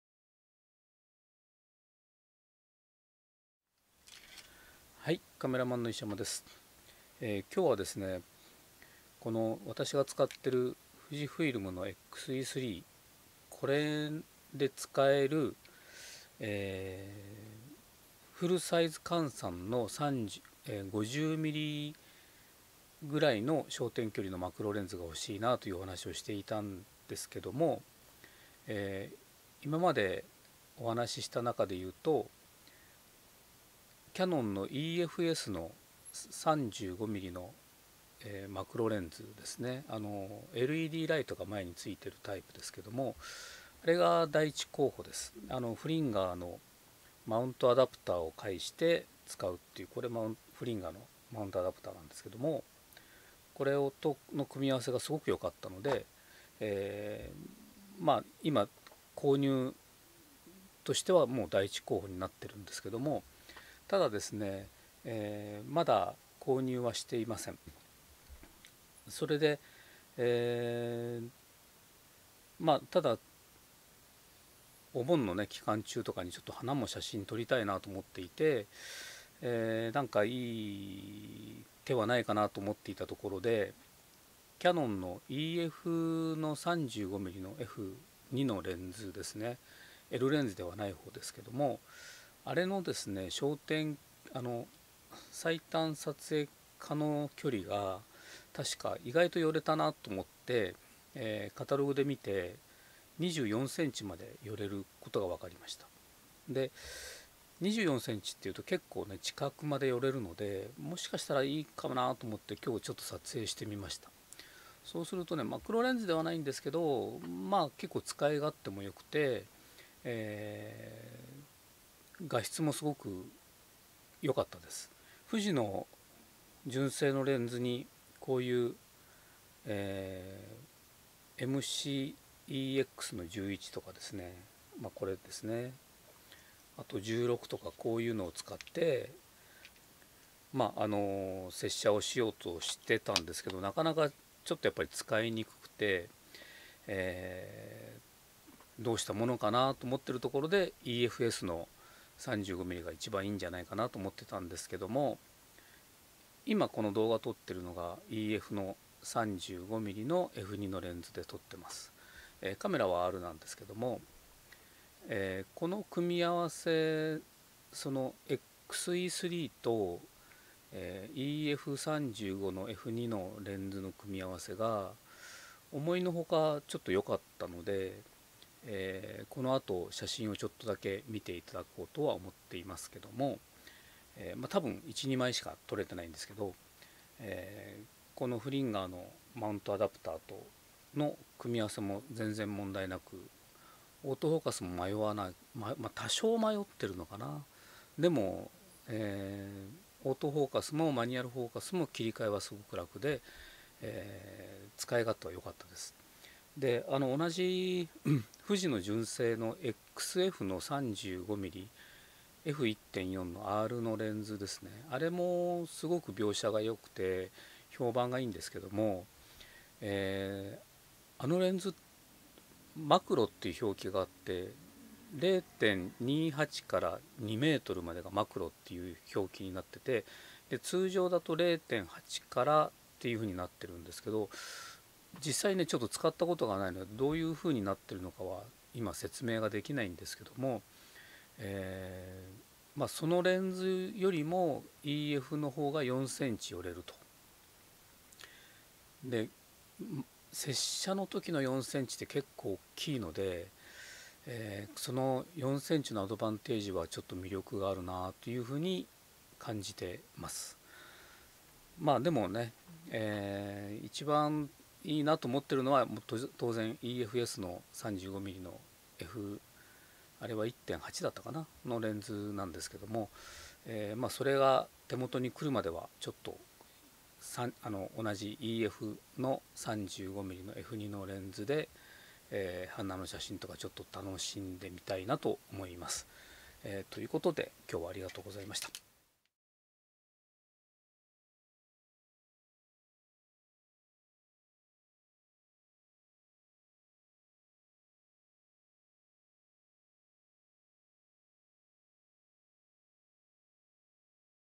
CanonEF35mmF2+FUJIFILM X-E3の組み合わせで花を撮りました。
CanonEF35mmF2のレンズを使ってFUJIFILM X-E3で花を撮りました。